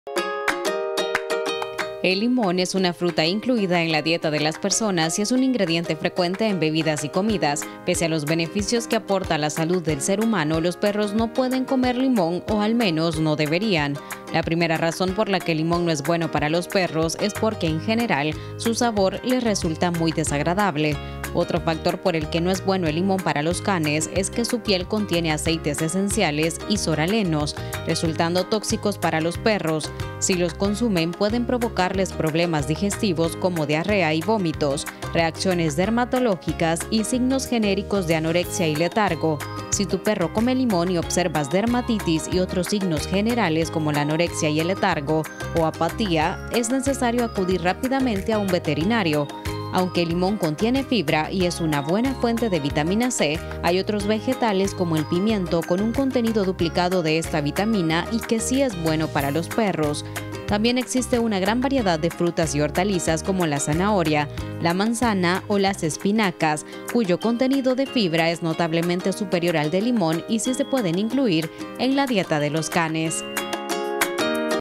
DeMujerAMujer | ¿Los perros pueden comer limón?
Aunque nosotros seamos amantes del limón, no quiere decir que nuestros perros también lo sean. Según expertos los perros no deben comer limón porque puede resultar dañino para ellos. Le contamos las razones.